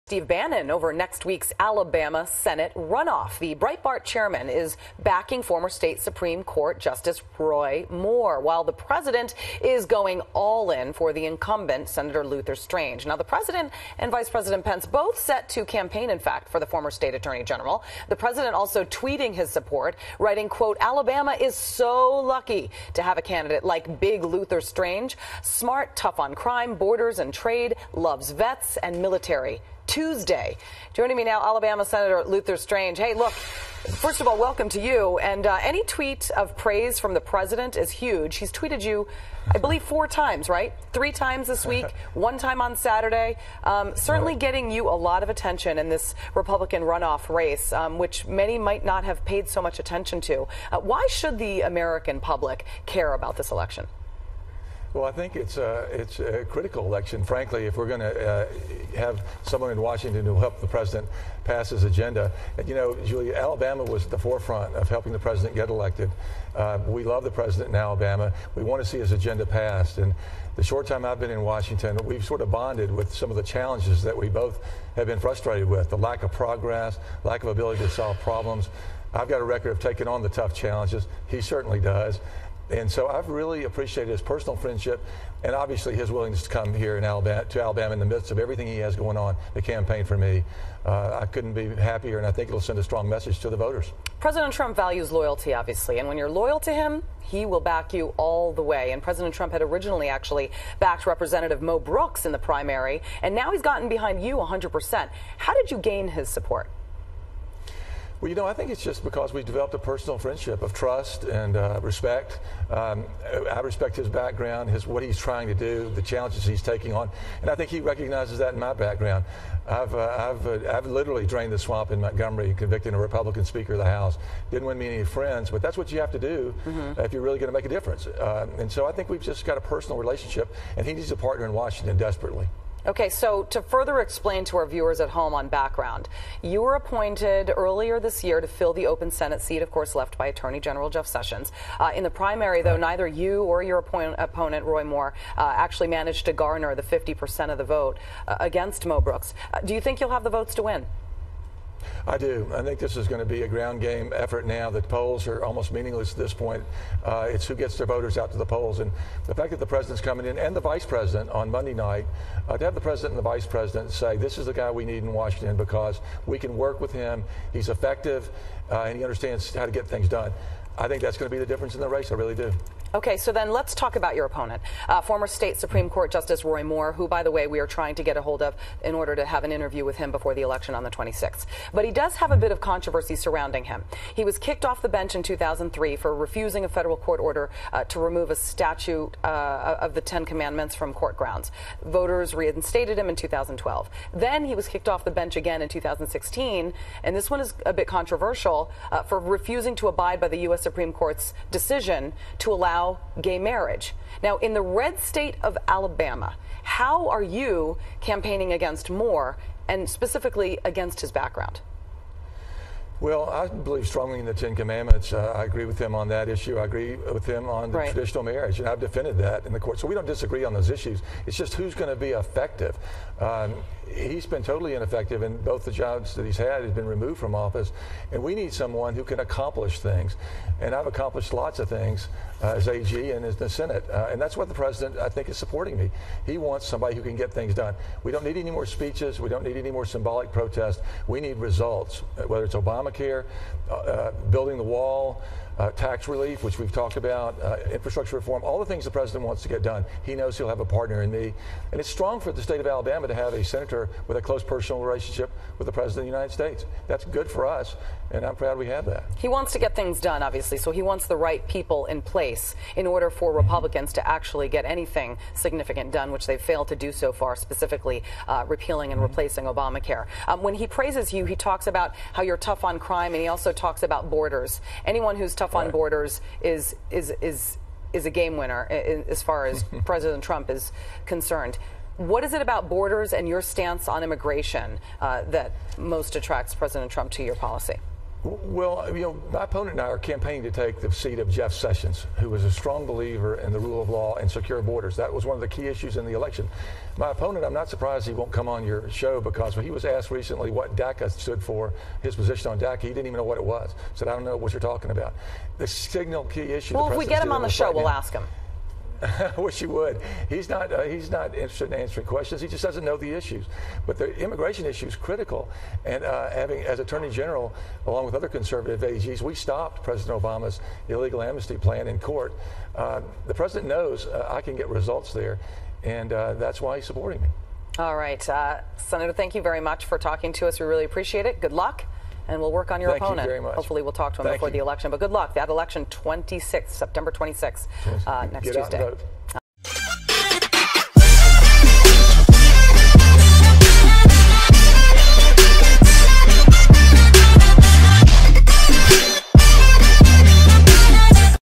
BREAKING NEWS 9/21/17: Luther Strange confident he'll win runoff with Trump support
Breaking news today 9/21/17 | Latest update news !

BREAKING NEWS 9/21/17:  Luther Strange confident he'll win runoff with Trump support.Happening Now',Alabama Republican Senate candidate makes case for seat vacated by Jeff Sessions

Follow Primary US News Channel to   Get Latest USA News Today